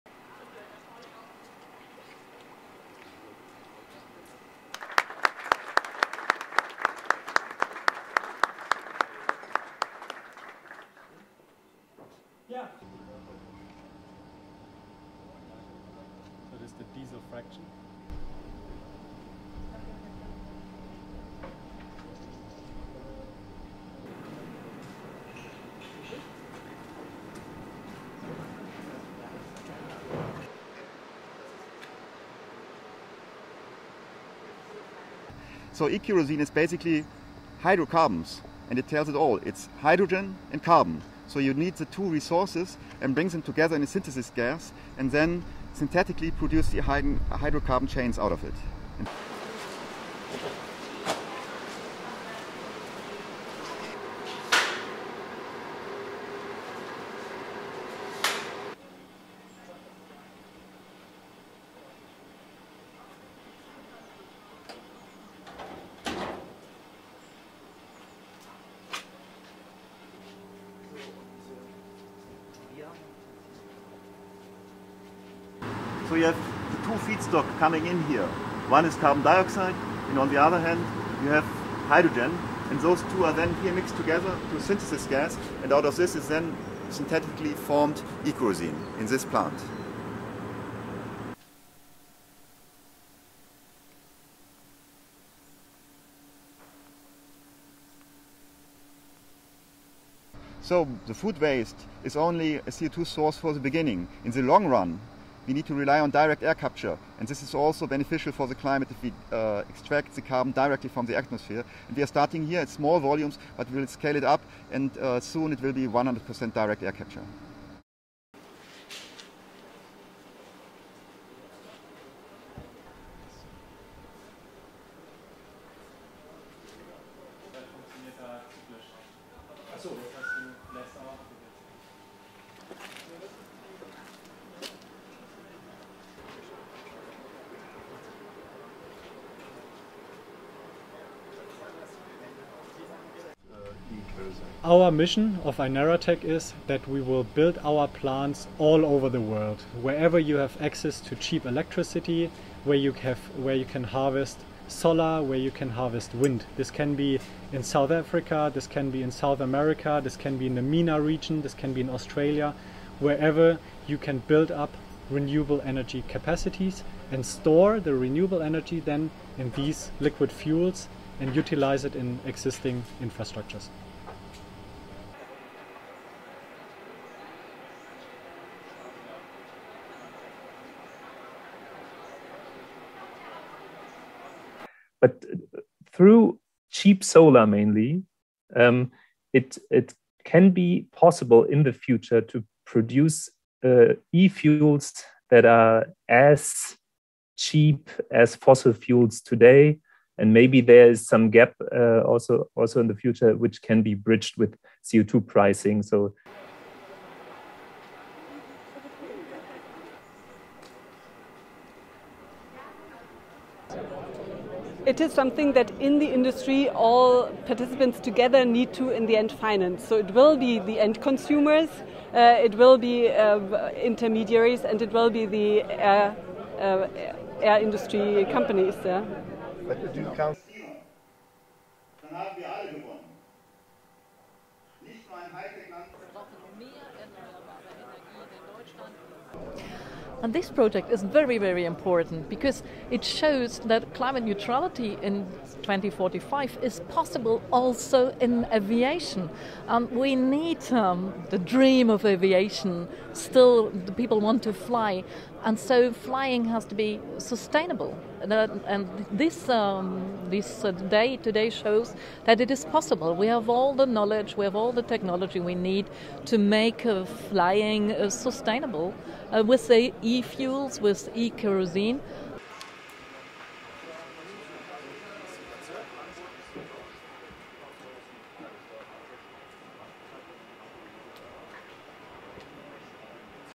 CO2-neutral kerosene could fuel airline industry
(5 Oct 2021) LEAD IN:
A new plant in Germany which aims to cut the carbon footprint of the aviation industry has opened in Werlte.
The plant makes synthetic kerosene - a carbon-neutral means of fuelling aircraft.
Germany's environment minister says the project will mean climate neutrality by 2045 is possible for aviation.

STORY-LINE:
German officials on Monday (5 October 2021) unveiled what they said is the world's first commercial plant for making synthetic kerosene, touted as a climate-friendly fuel of the future.
Aviation currently accounts for about 2.5% of worldwide emissions of carbon dioxide, a greenhouse gas that contributes to global warming.
While other forms of transportation are increasingly being electrified, the challenge of making large, battery-powered planes is formidable.
Experts say e-fuels can help solve the problem by replacing fossil fuels without major technical modifications to the aircraft.
The facility in Werlte, near Germany's northwestern border with the Netherlands, will use water and electricity from four nearby wind farms to produce hydrogen.
In a century-old process, the hydrogen is combined with carbon dioxide to make crude oil, which can then be refined into jet fuel.
Atmosfair, a German non-profit group is behind the project
"The e-kerosene is basically hydrocarbons, and it tells it all, it's hydrogen and carbon. So you need the two resources, and it brings them together in a synthesis gas and then synthetically produced hydrocarbon chains out of it," says Dietrich Brockhagen, Atmosfair's chief executive.
Atmosfair says burning that synthetic kerosene releases only as much CO2 into the atmosphere as was previously removed to produce the fuel, making it "carbon neutral."
The amount of fuel that the plant can produce beginning early next year is modest: just eight barrels a day, or about 336 gallons of jet fuel.
That would be enough to fill up one small passenger plane every three weeks.
By comparison, total fuel consumption of commercial airlines worldwide reached 95 billion gallons in 2019, before the pandemic hit the travel industry, according to the International Air Transport Association.
But Atmosfair, says its purpose is to show that the process is technologically feasible and, once it is scaled up and with sufficient demand, economically viable.
Falko Ueckerdt is a senior researcher and team leader at the Potsdam Institute of Climate Impact Research who is not involved with the project.
"Through cheap solar, mainly, it can be possible in the future to produce e-fuels that are as cheap as fossil fuels today," he says.
Initially the price of synthetic kerosene produced in Werlte will be far higher than that of regular jet fuel, though Atmosfair won't divulge how much it will be charging its first customer, the German airline Lufthansa.
However Atmosfair's chief executive, Dietrich Brockhagen, says a price of 5 euros ($5.80) per litre (0.26 gallons) is possible.
That's still several times what kerosene currently costs, but Atsmofair is banking on carbon taxes driving up the price of fossil fuels, making his product more competitive.
Dorothea von Boxberg, the head of Lufthansa Cargo, says the additional cost will have to be borne by all involved.
"It will be in the end consumers, it will be intermediaries and it will be the air industry companies."
Additionally, authorities at the national and European level are putting in place quotas for the amount of e-fuel that airlines will have to use in future.
That will create demand, making it more attractive to invest in bigger and better plants.